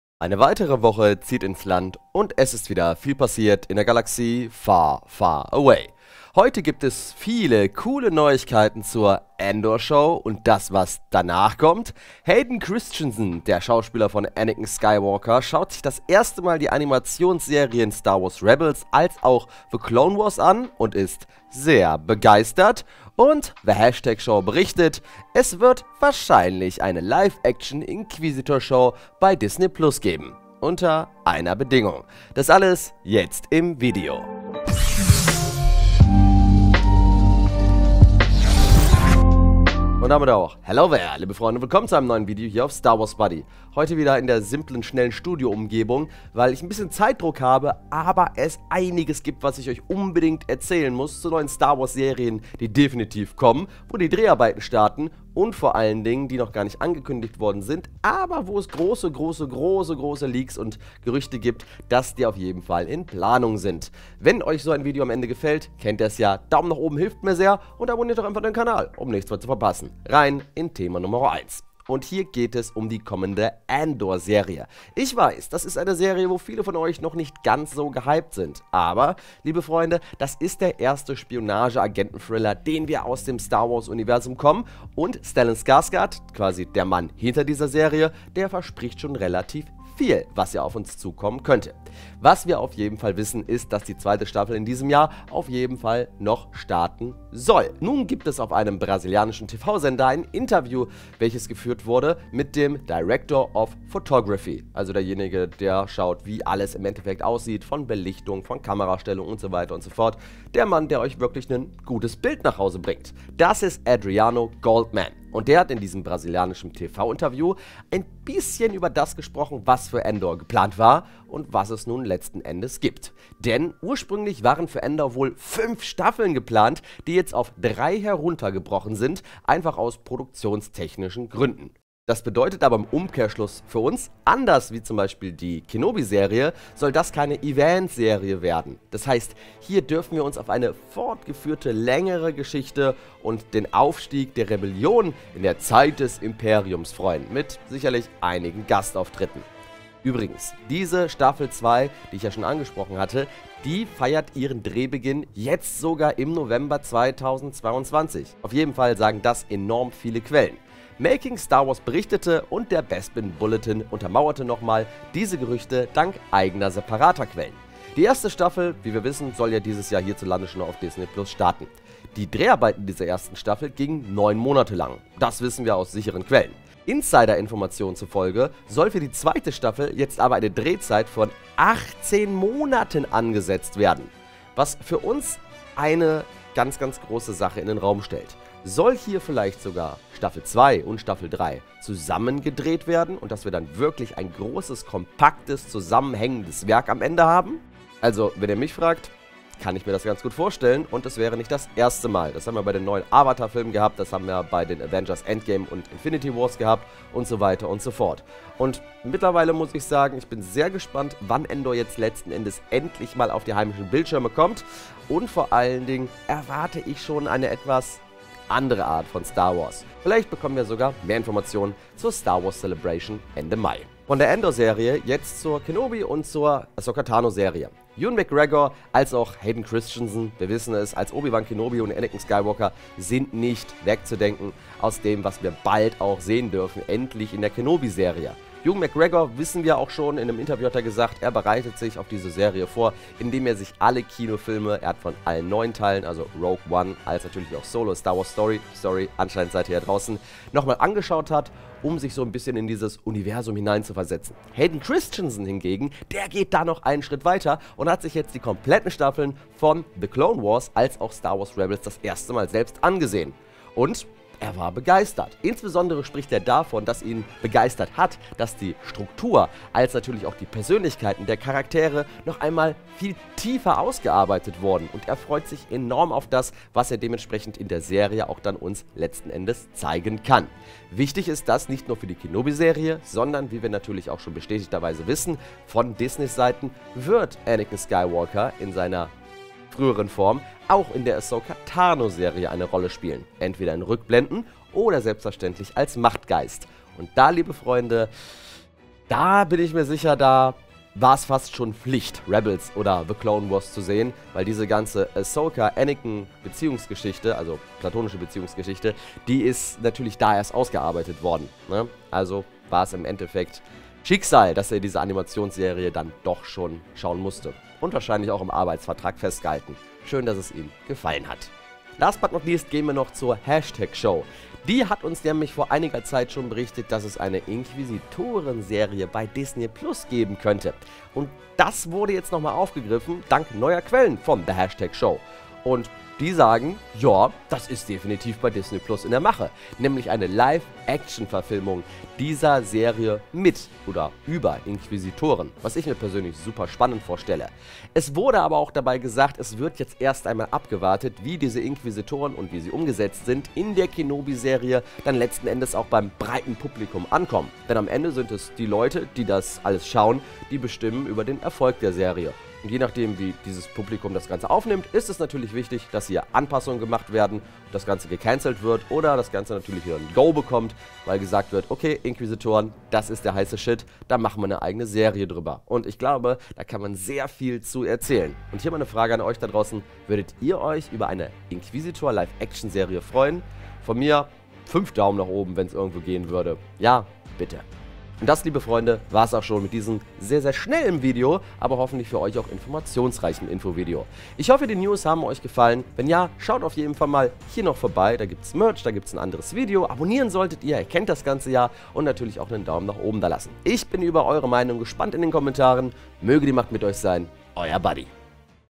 INQUISITOR Serie auf Disney+ geplant | Andor mit 3 Staffeln & Anakin in Ahsoka | STAR WARS News
In den heutigen News gehts rund - Angeblich soll eine Inquisitor Serie für Disney+ geplant sein - je nachdem wie die Kenobi Serie beim Publikum ankommt . Außerdem sind mehreren Staffeln für Andor nach Release geplant & Anakin Skywalker Schauspieler Hayden Christensen bereitet sich mit STAR WARS Rebels & The Clone  Wars auf seine Auftritte in Obi Wan Kenobi & der Ahsoka Tano Serie vor.

Sämtliche gezeigten Star Wars Film & Serienmaterial sind Eigentum von Disney und Lucasfilm Ltd.